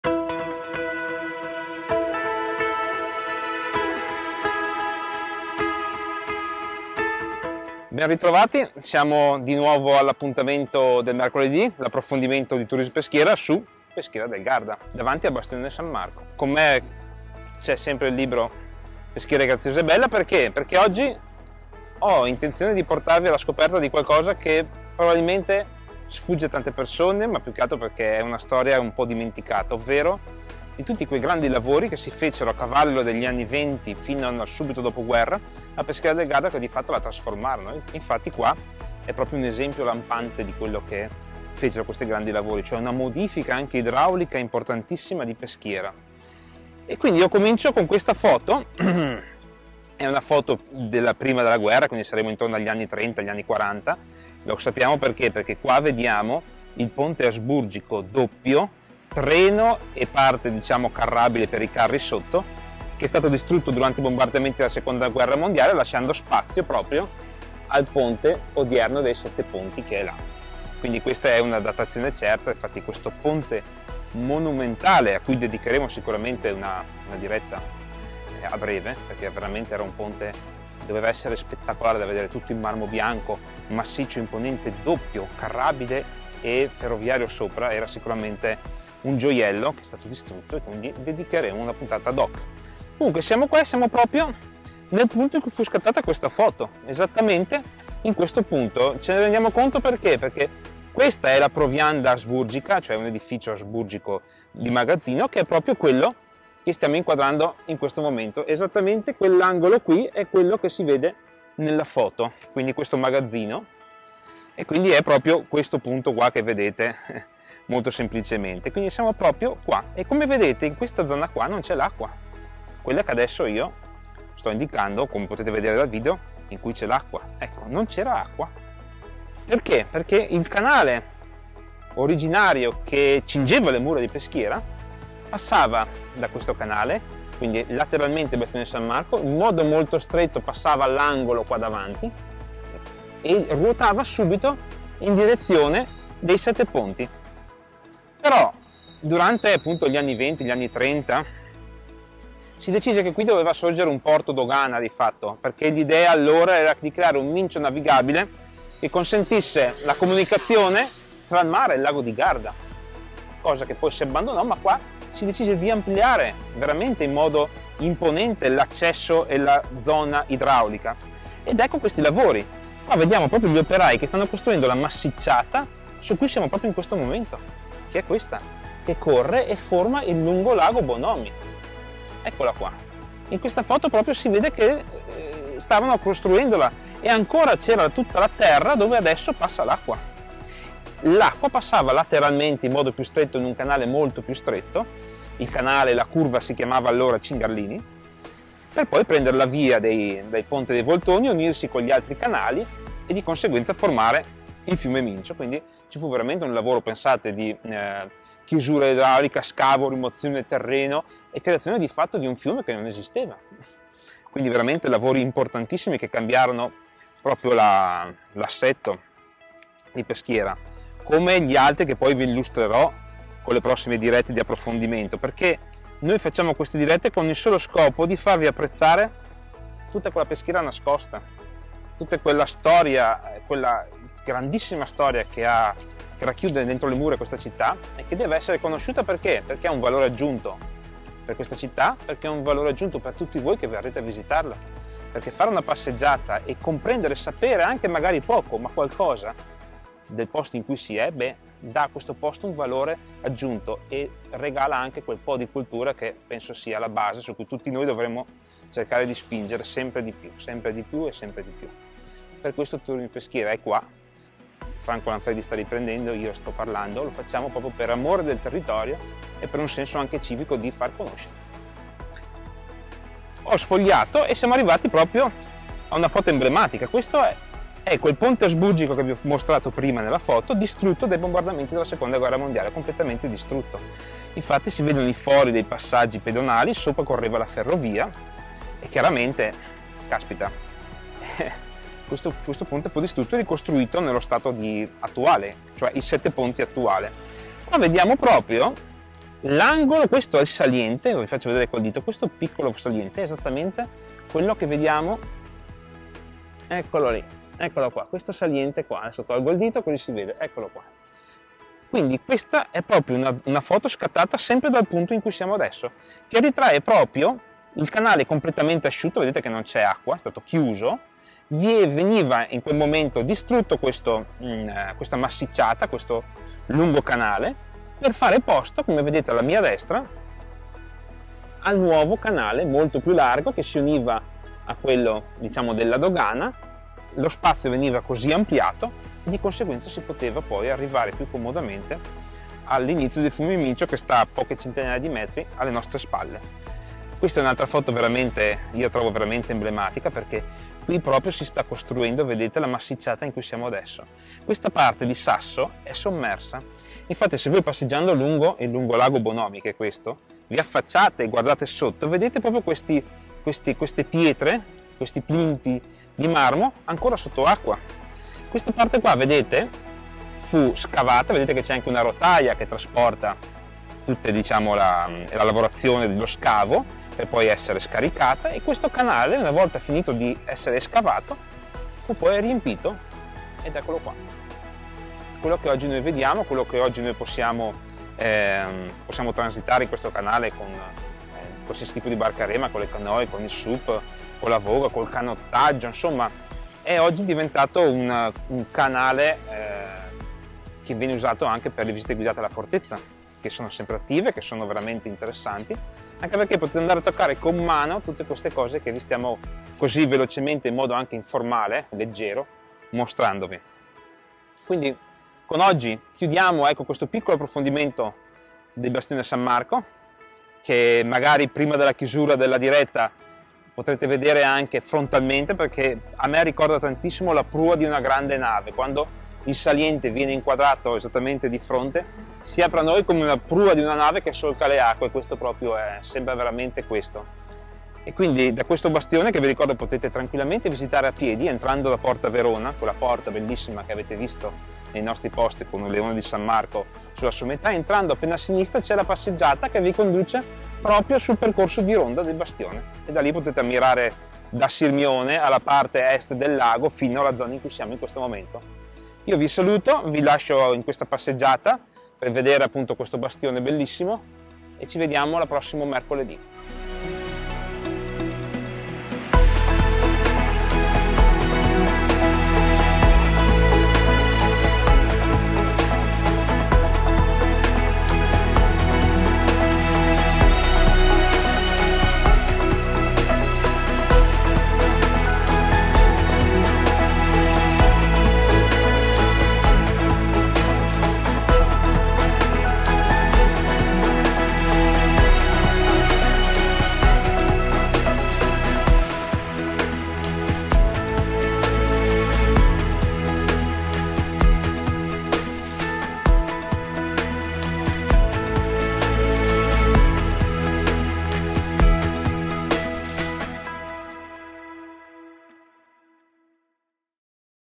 I Grandi Lavori sui Canali di Peschiera del Garda. Bastione San Marco.
Eccoci ad un nuovo appuntamento con gli approfondimenti del mercoledì. Oggi vi raccontiamo dei grandi lavori che, a partire dagli anni '20 del '900, hanno cambiato la fisionomia dei canali e lo scorrere delle acque del Lago di Garda verso il Fiume Mincio.